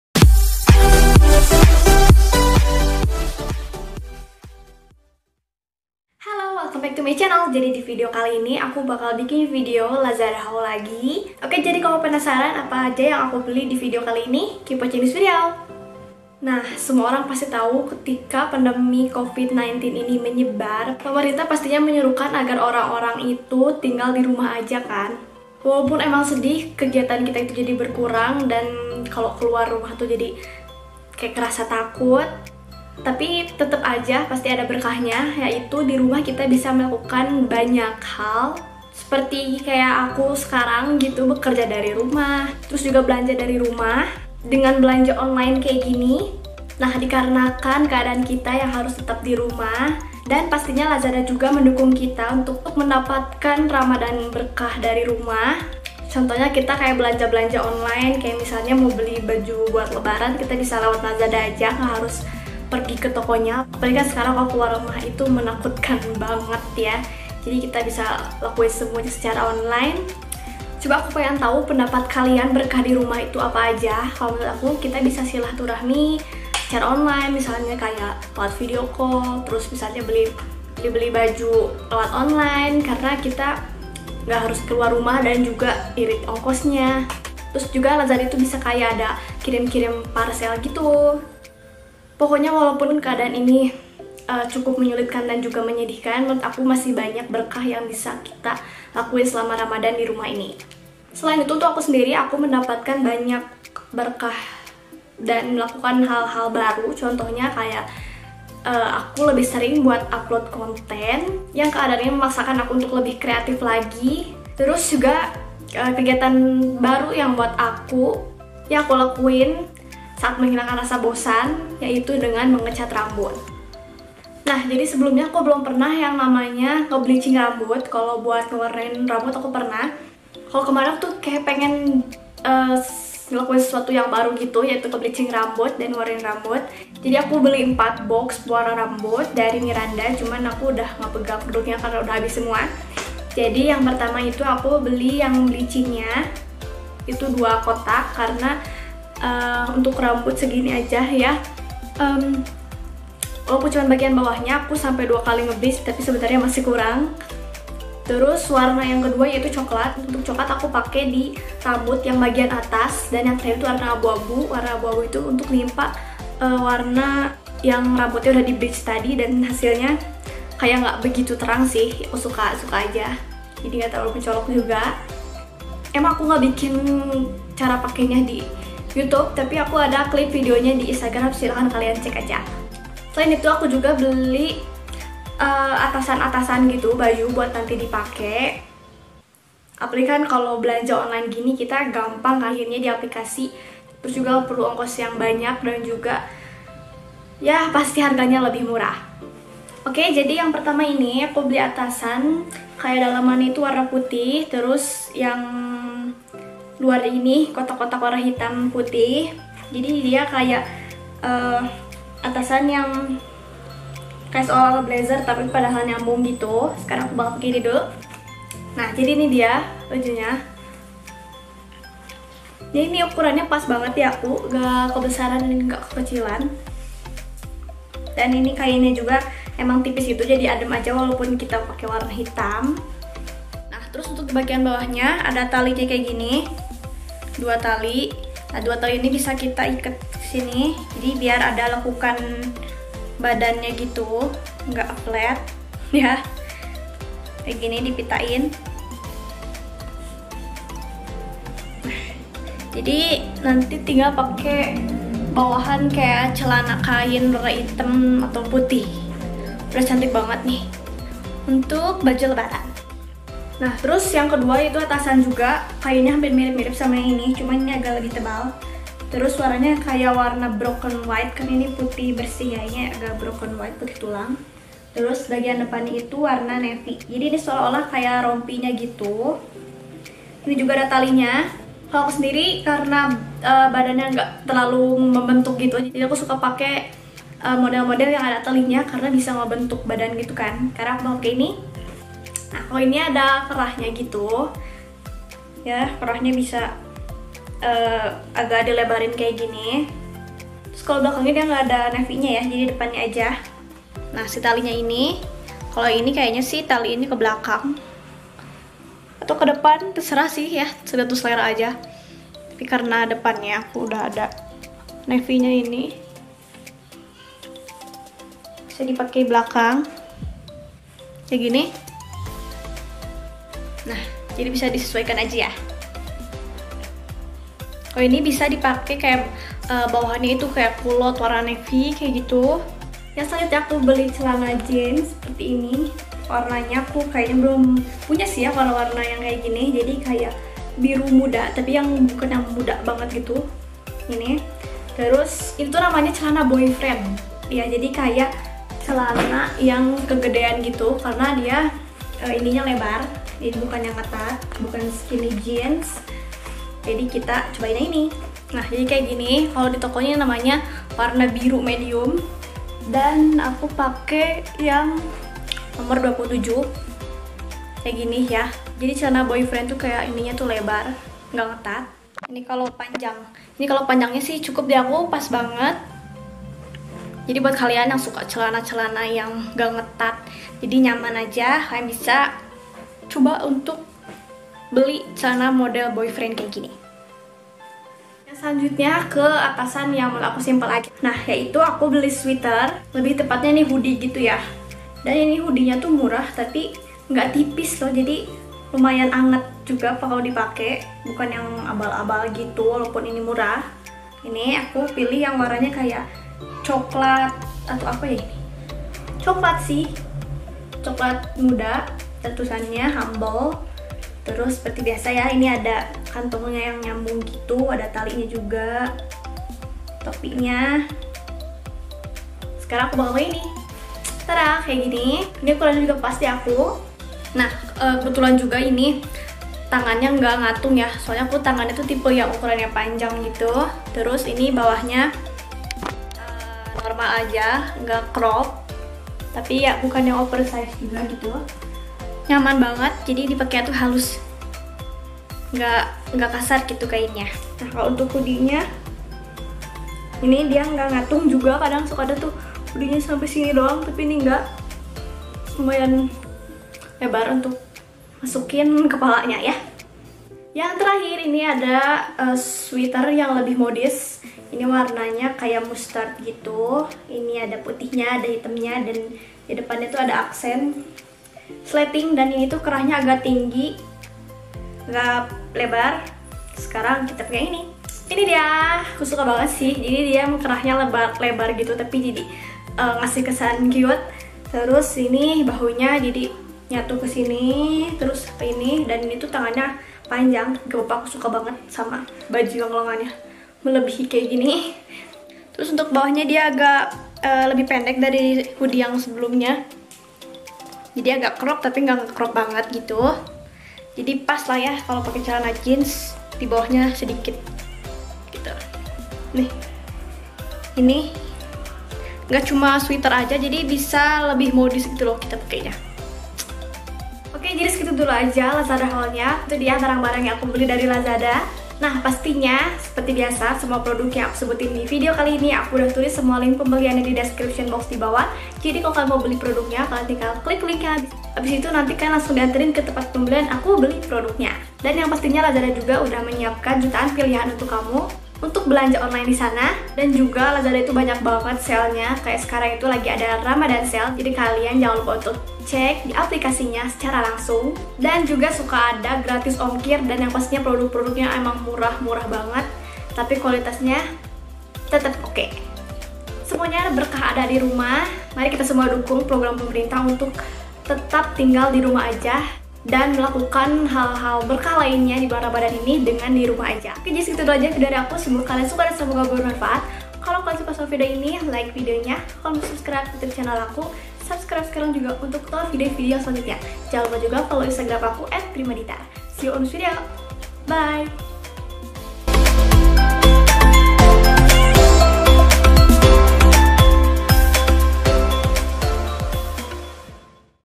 LAZADA HAUL !!! Belanja Baru Lebaran Murah + Warnain Rambut Saat dirumahaja | Primadita R
💕About Me 💕
- Video Editor? Kinemaster Pro
- Thumbnail Photo Editor? PixelLab
- Bogor, Indonesia.